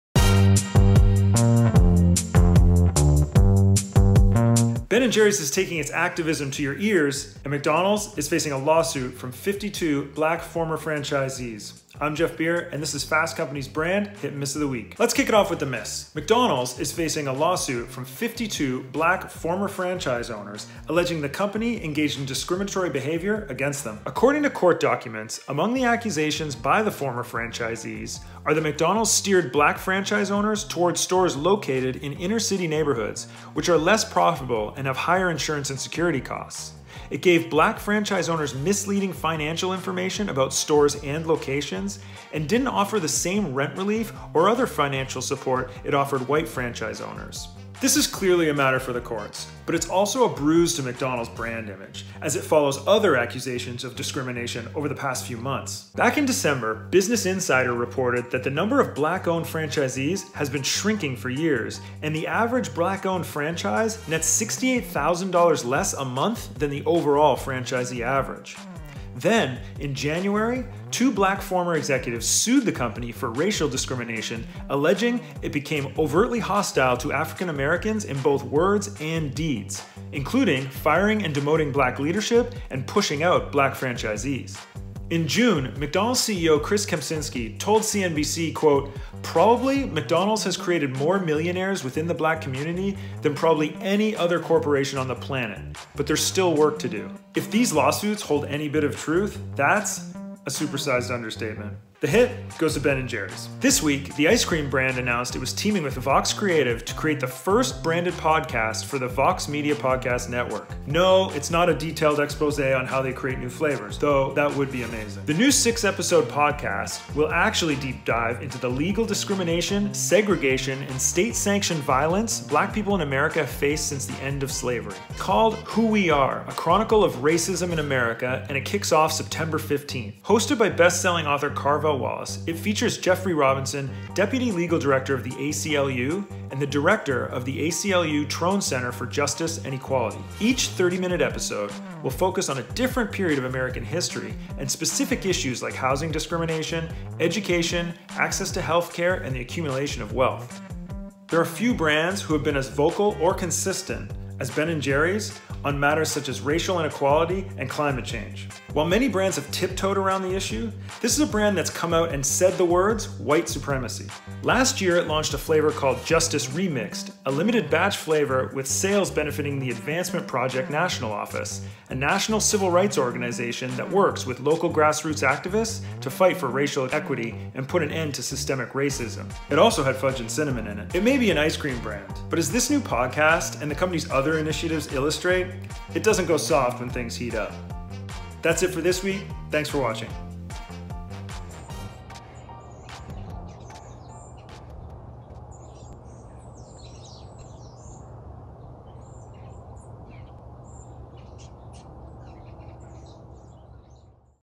McDonald’s $1 Billion Lawsuit vs Ben & Jerry’s Antiprejudice Podcast: Brand Hit and Miss of the Week
McDonald’s gets sued for discriminating against Black franchisees, and Ben & Jerry’s adds to its activism with a new podcast about racism in America.